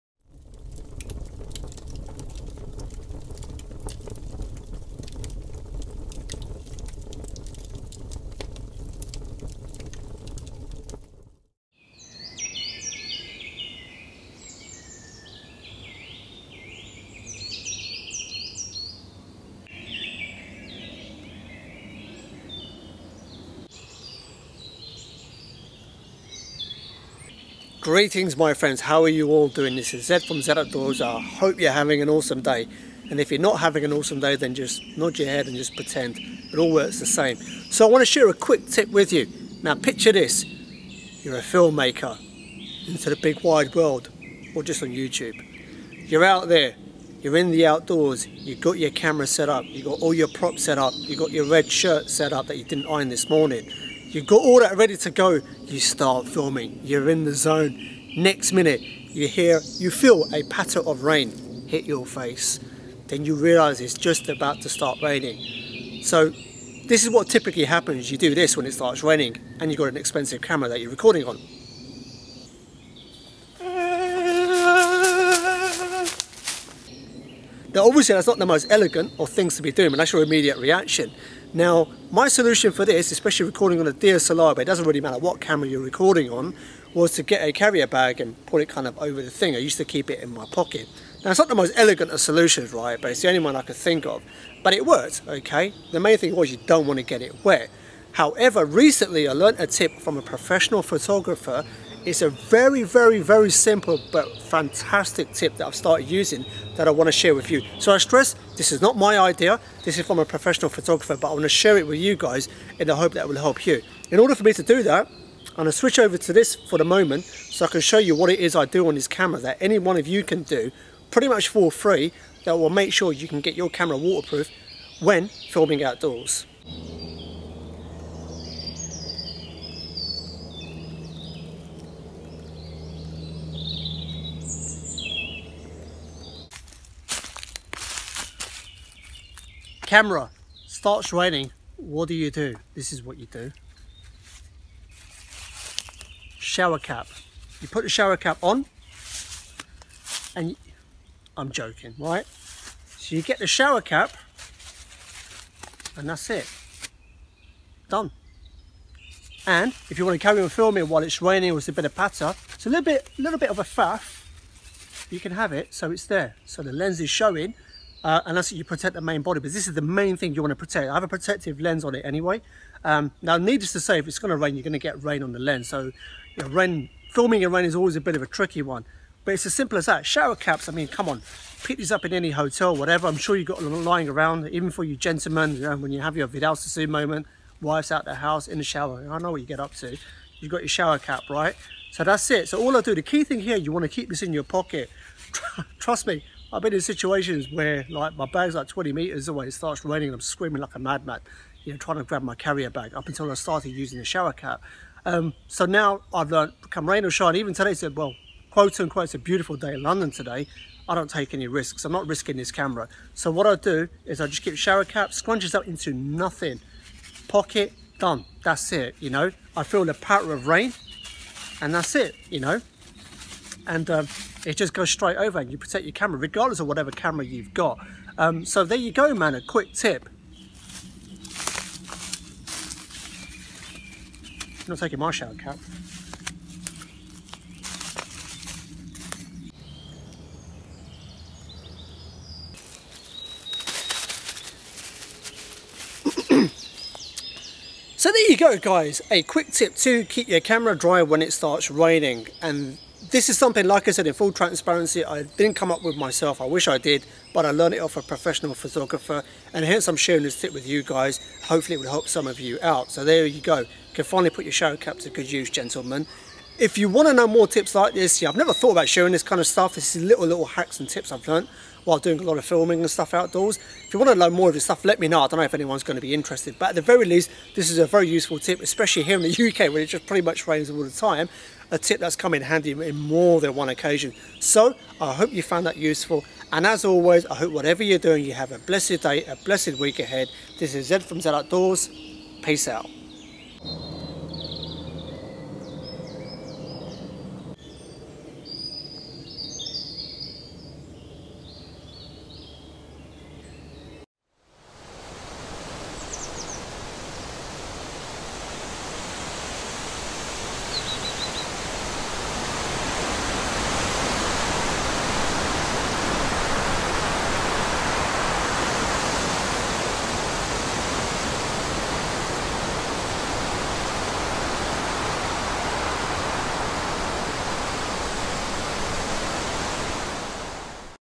Quick Tip For Protecting Your Camera In The Rain
[This video is viewable in 'Full HD']
[Please click on the 'Show More' tab for more information and important links]

I've been filming outdoors for over two years now in the unpredictable British weather and along the way i've picked up a few tips

This tip I learnt from a professional photographer and so thought i'd share it with you as its great for when it suddenly starts raining and you want to quickly protect your camera

As always I appreciate you watching

Peace

Zed